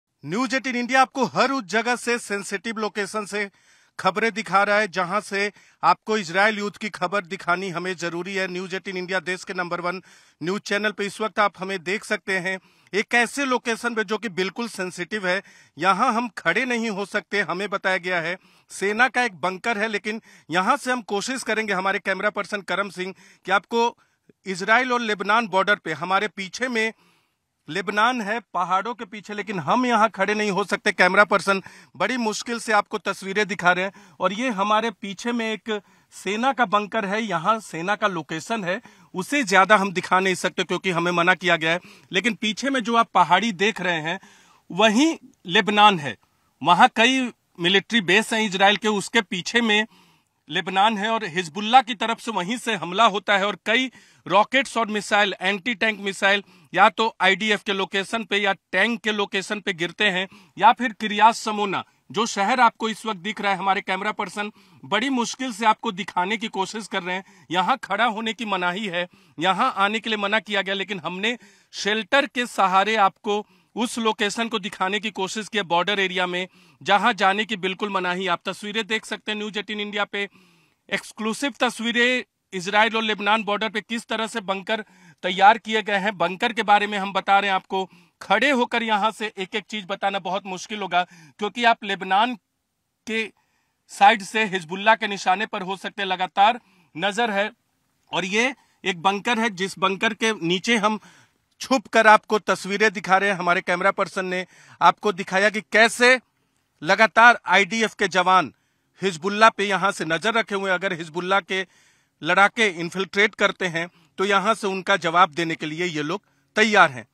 Hezbollah attack on Israel : Lebanon Border पर IDF के बंकर्स से Exclusive | Nasrallah | N18V
लेबनान बॉर्डर पर IDF के बंकर्स से देखिए News 18 इंडिया की एक्सक्लूसिव रिपोर्ट

इजराइल और लेबनान बार्डर पर हिजबुल्लाह के हमले से बचने और जवाबी कार्रवाई के लिए बने आईडीएफ के बंकेर्स लोकेशन से एक्सक्लूसिव रिपोर्ट

कैसे इजराइल-लेबनान बॉर्डर के आर्मी के संवेदनशील लोकेशन से आपको दिखा रहा है बॉर्डर का हाल

पहाड़ के ऊपरी हिस्सा से बॉर्डर शहर किर्यात समोना को दिखाया है और लेबनान बॉर्डर को दिखाते हुए
Watch News 18 India's exclusive report from IDF bunkers on the Lebanon border.

Exclusive report from IDF bunkers location built to protect and retaliate against Hezbollah attacks on Israel and Lebanon border.

How the army is showing you the condition of the border from a sensitive location on the Israel-Lebanon border.

View from the top of the mountain showing the border town of Kiryat Samona and the Lebanon border